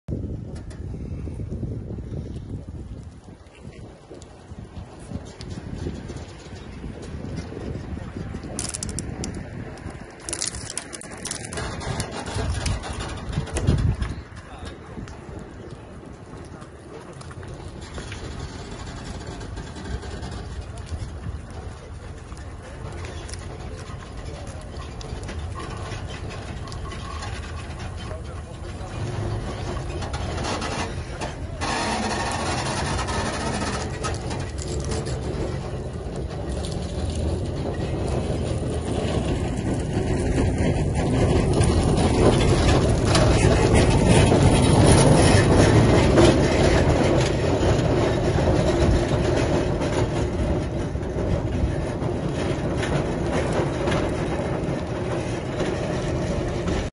Timisoara : probe pe linia de tramvai care duce la Shopping City
Probe facute de STPT pe linia de tramvai care duce catre complexul comercial Shopping City

Citeste ultimele stiri, informatii si noutati despre Timisoara! Fii la curent cu ce se intampla pe Bega, fii informat!